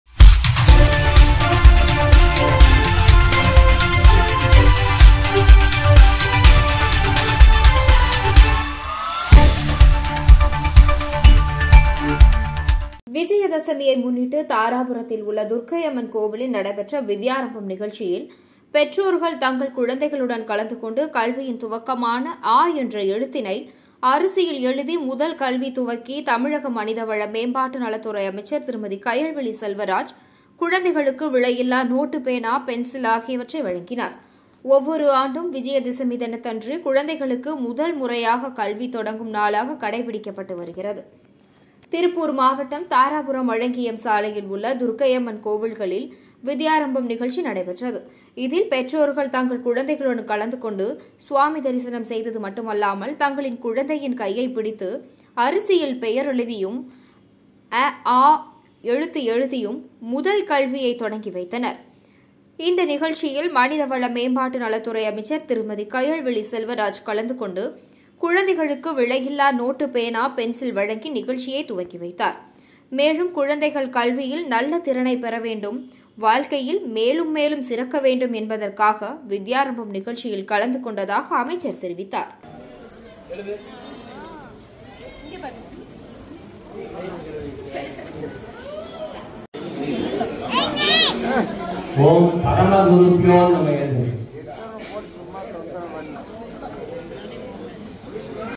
விஜயதசமியை முன்னிட்டு துர்க்கை அம்மன் கோவிலில் நடைபெற்ற வித்யாரம்பம் நிகழ்ச்சி
விஜயதசமியை முன்னிட்டு  துர்க்கை அம்மன்
 கோவிலில் நடைபெற்ற வித்யாரம்பம் நிகழ்ச்சி